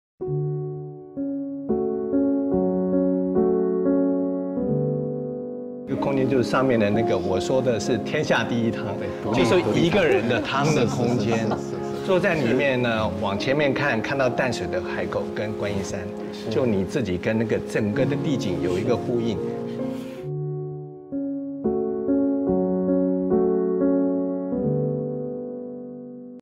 【大隱豐盈海】行雲山水安慢自在！美學首席 陳瑞憲 千坪殿堂級公設
紅樹林水岸第一排，無際水岸至高巨景，天人合一片刻留白！生活是一幅動態風景，每一刻都要追求不凡而存在，以不凡超脫日常。【大隱豐盈海】戶戶27米大面寬，超大陽台無界延伸，收藏淡水河、觀音山遼闊大視野。